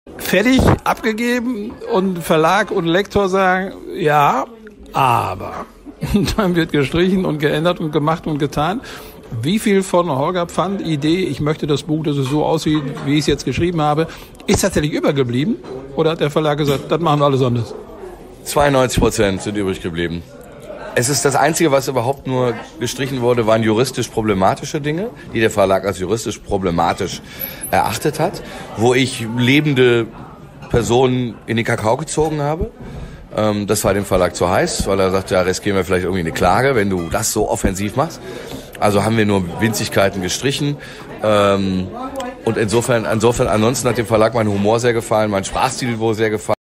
Folge 19: Die andere Seite eines Fußballreporters
Seit gestern online in Bosses Bundesliga Blog :

Thema: Die andere Seite eines Fußballreporters. Gast: Holger Pfandt

Der bekannte Fußballkommentator von Sky Sport DE hat sich als Autor versucht - mit Erfolg! Sein Ausflug in die Belletristik hat das Erstlingswerk Altobelli Deutschland ans Licht gebracht. Der Düsseldorfer zeigt sich hier von seiner witzigen, satirischen Seite.

Zu hören hier
Über die kostenlose App „Football was my first love“
Über die kostenlose App von RTL „Audio now“